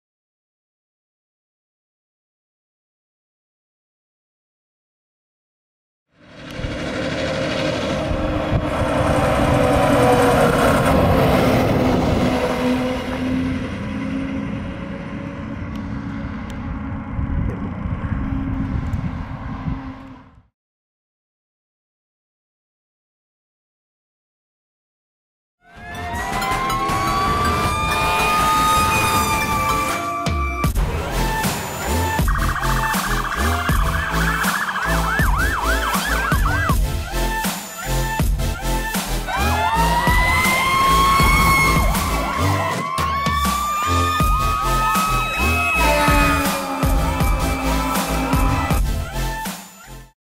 Harleysville Community Fire Company Tanker 89 Responding 3/6/22
Harleysville Community Fire Company Tanker 89 Responding in Upper Salford Township to a working building fire in Montgomery County PA.
Date: 3/6/22